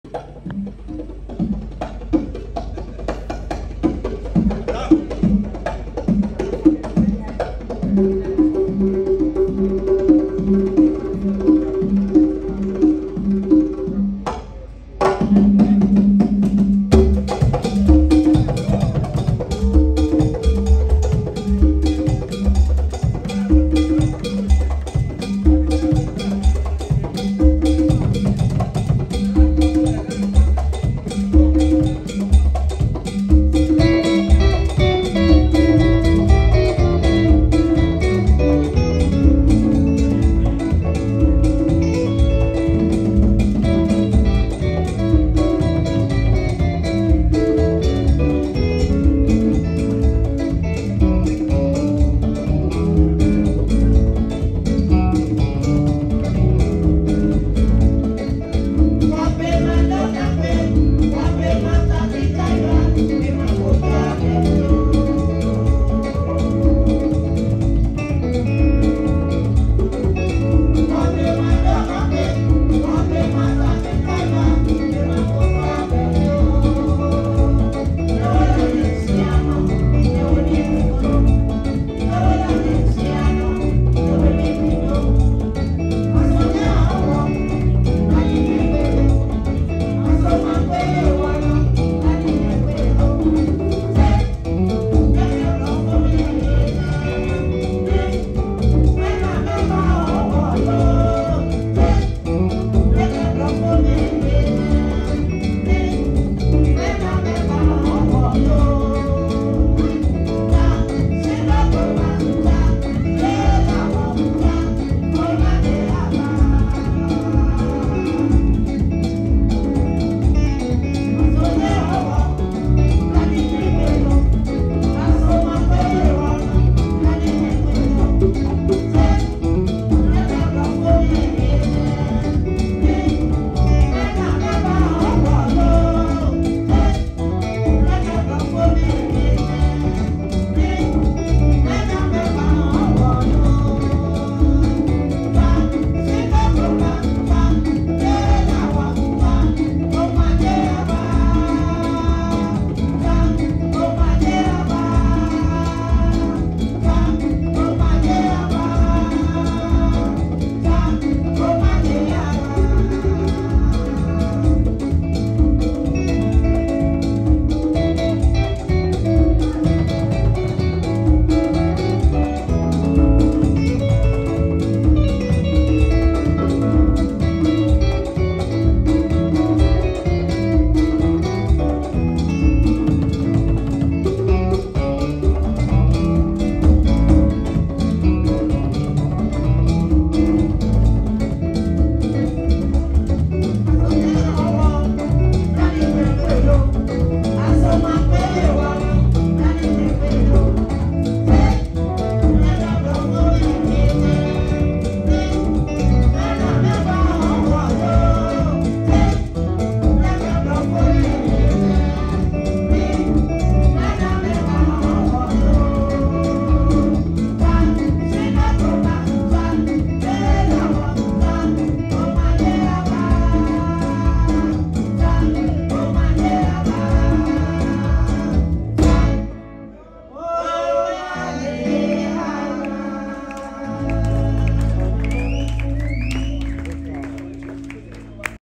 Legendary Naa Amanua of Wulomei performance at Jamestown Boutique (PART 2)
Mary Naa Amanua Dodoo, who is also recognized as Naa Amanua, stands as a Ghanaian artist specializing in Ga folklore music. She gained prominence as the lead female vocalist of Wulomei, a Ghanaian musical ensemble established in 1973. In 2018, Naa Amanua was honored with the Vodafone Ghana Music Awards Lifetime Achievement accolade.